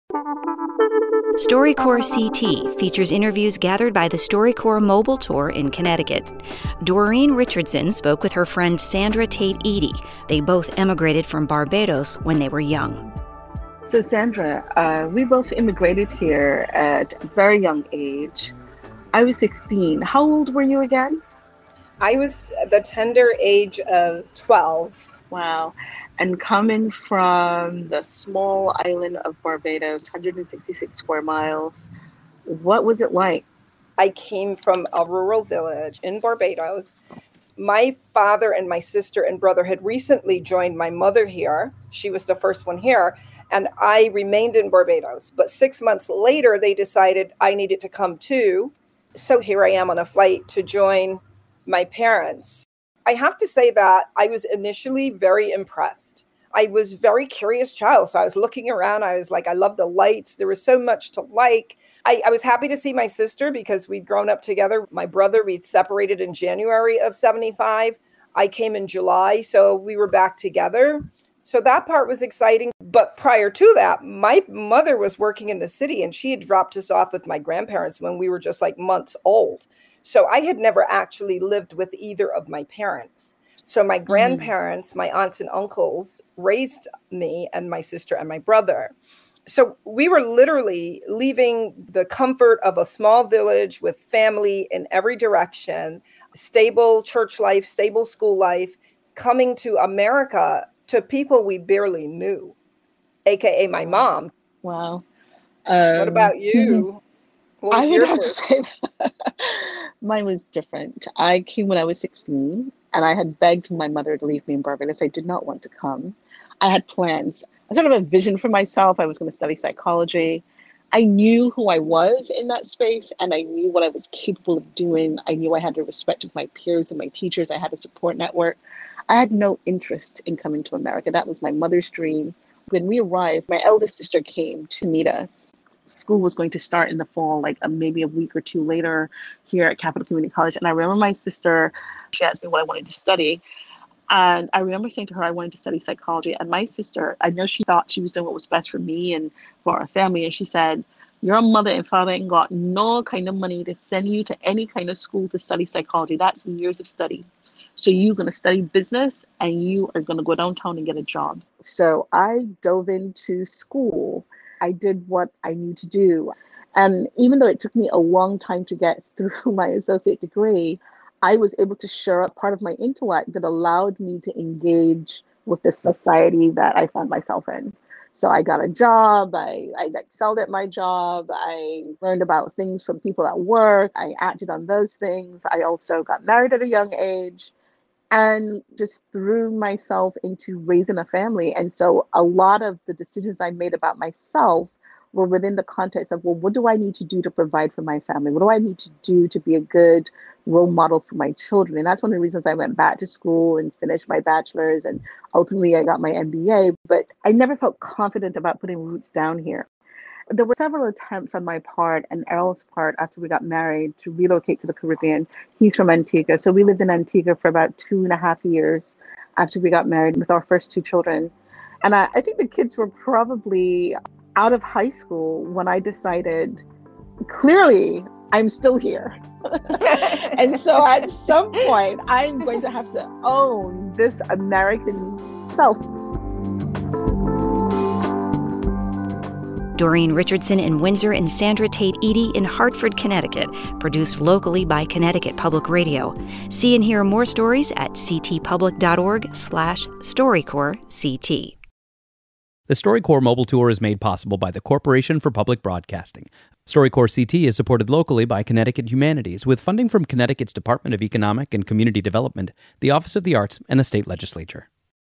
Coming from 166 square miles
Sandra and Doreen both immigrated from Barbados to the U.S. in their youth and share two share very different experiences of coming to North America. Doreen also explains how she never felt confident putting roots down here.

“I have to say I was initially very impressed. I  was a very curious child, so I was looking around, I was like, I love the lights. There was so much to like.” - Sandra

“Clearly, I’m still here. And so, at some point, I’m going to have to own this American self.” - Doreen

W7Bv9J4JkH70C2XGbeFG